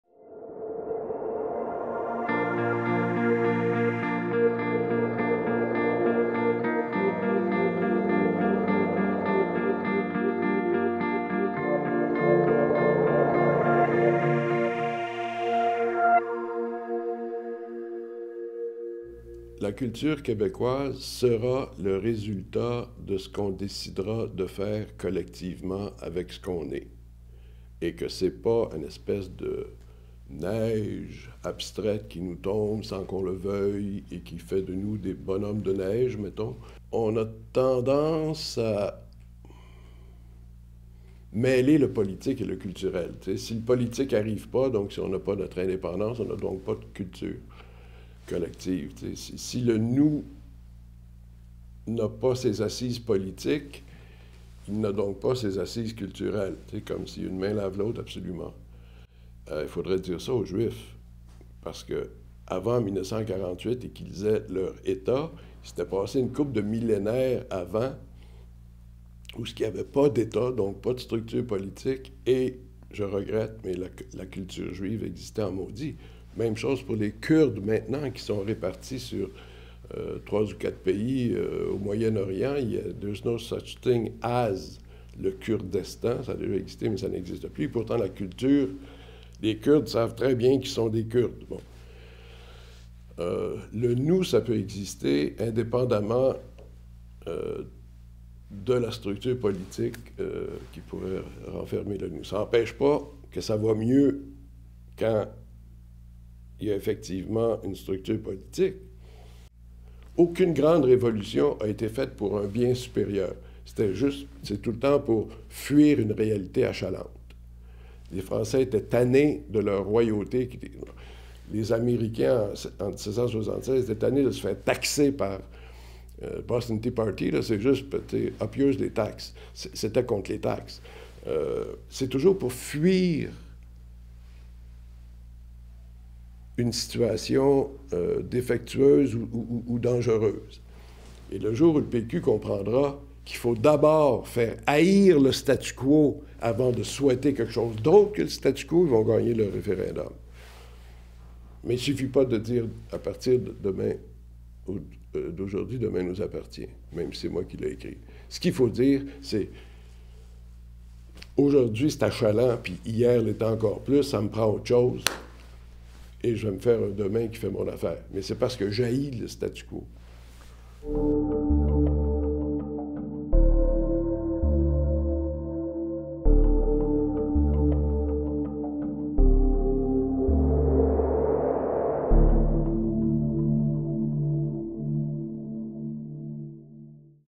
La spécificité québécoise
Stéphane Venne, auteur-compositeur-interprète, arrangeur, producteur et administrateur, croit que la culture québécoise sera le résultat de ce qu’on décidera de faire collectivement avec ce qu’on est.